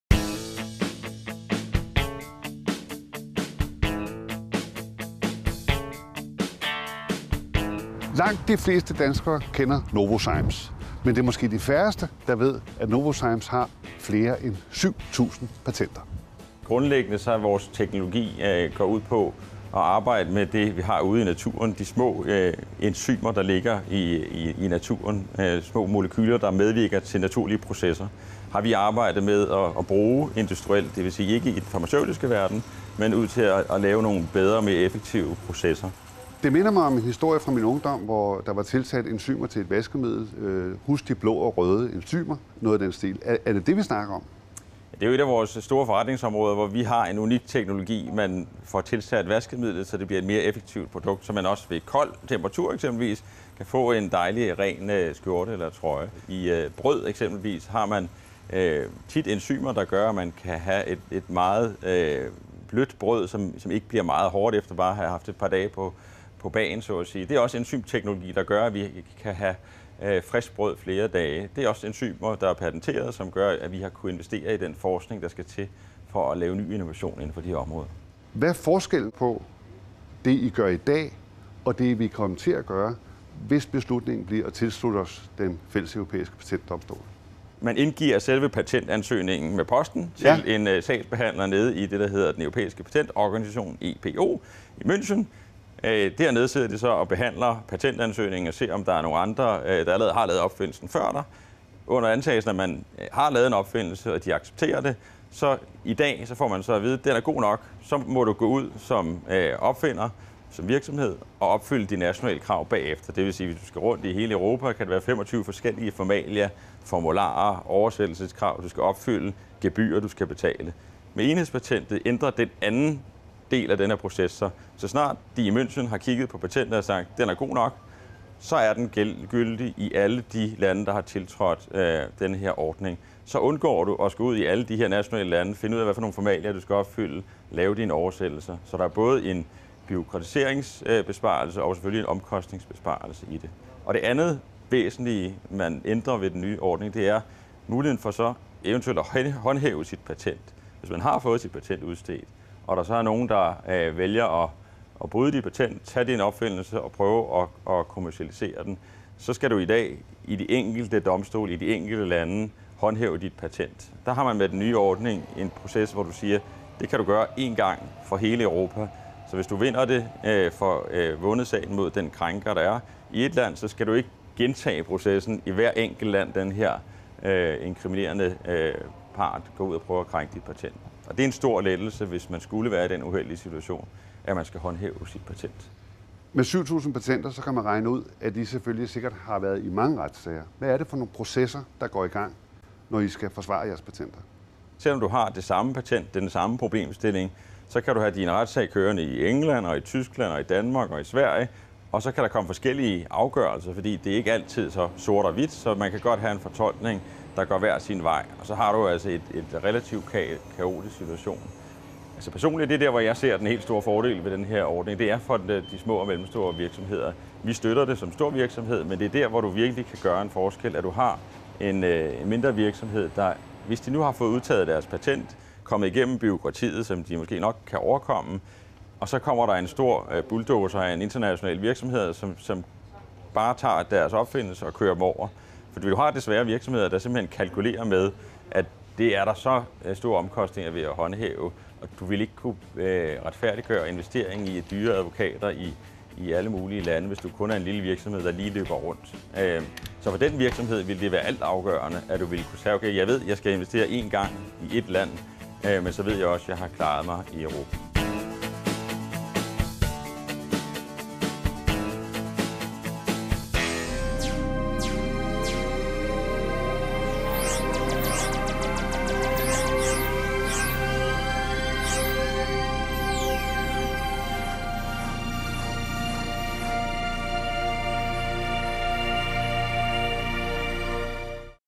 Novozymes - fra affald til benzin - et dansk patenteventyr
Novozymes er en verdensomspændende biotech-virksomhed med fokus på produktion af enzymer. Denne store danske virksomhed har en afdeling for patentering, der ledes af patentchef Thomas Batchelor. I dette program fortæller han om virksomheden og afdelingens arbejde.

Vært: Ole Stephensen

Medvirkende: Thomas Batchelor, patentchef, Novozymes
MH0753 Novozymes - fra affald til benzin - de fantastiske enzymer - et dansk patenteventyr (web)